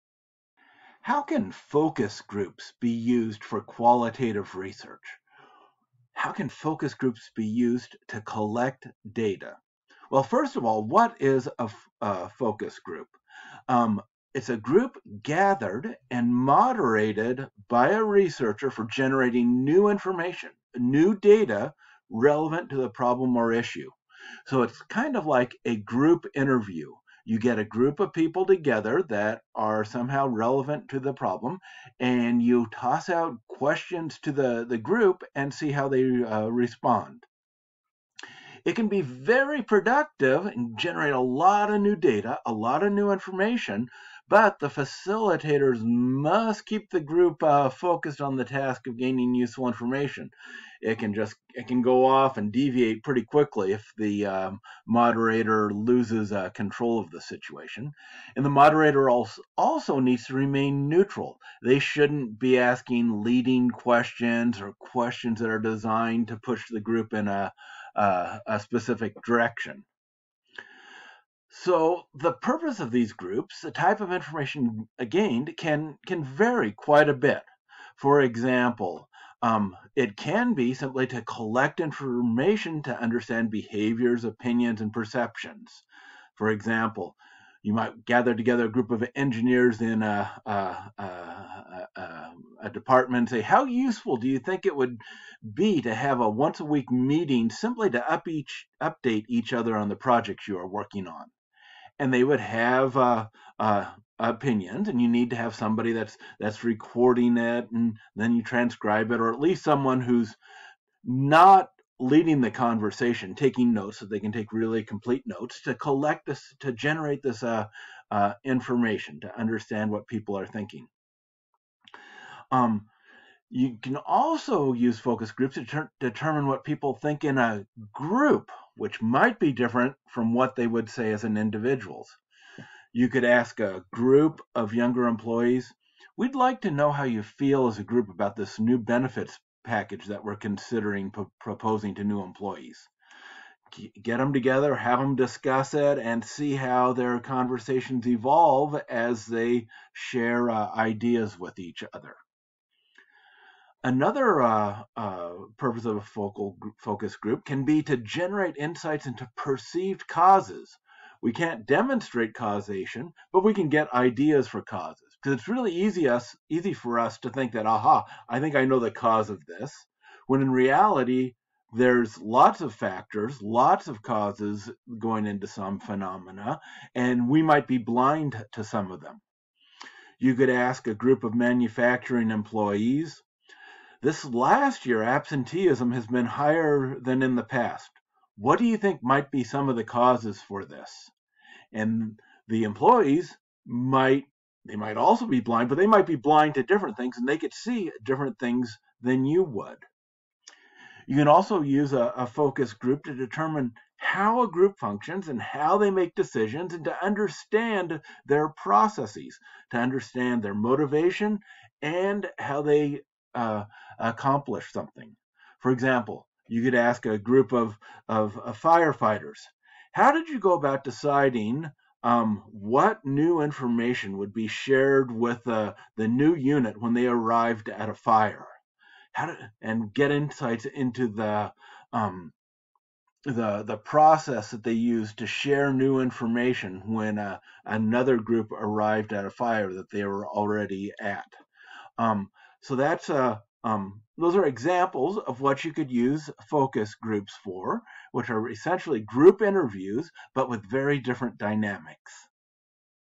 Why Use Focus Groups in Qualitative Research
A brief overview of focus groups.

The text comes from from Research Methods and Survey Applications by David R. Dunaetz. You can find a free PDF of this book by googling the title or you can buy a hard copy on Amazon.